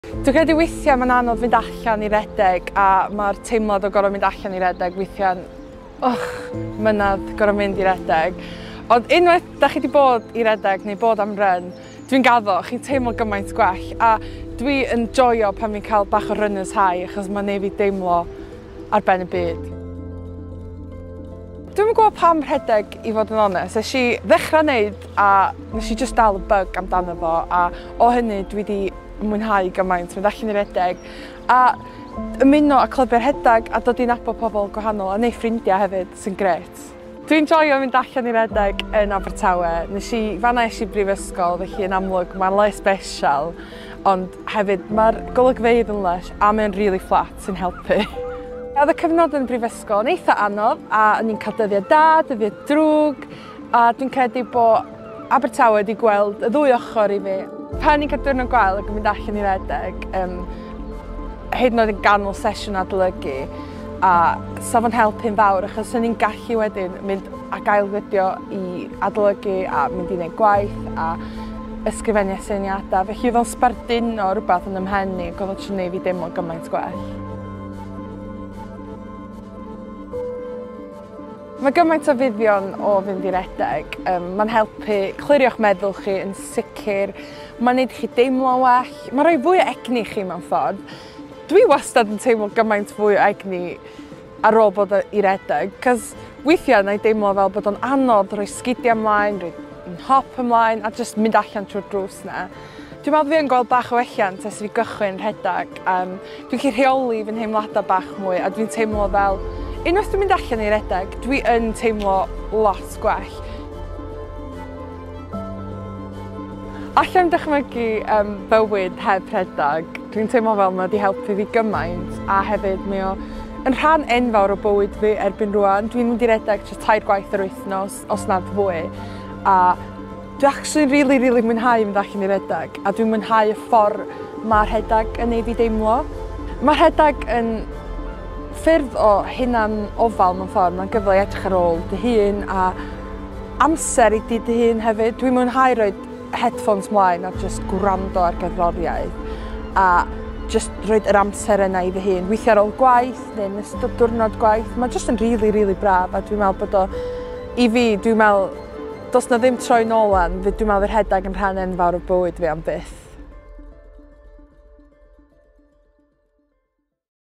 Er fy Lles: Rhedeg - Elen Jones
“Dwi actually yn rili mwynhau mynd allan i redeg a dwi'n mwynhau y ffordd ma' rhedeg yn gwneud i fi deimlo.”

Stori bersonol Elen o ffeindio llesiant ofewn Rhedeg…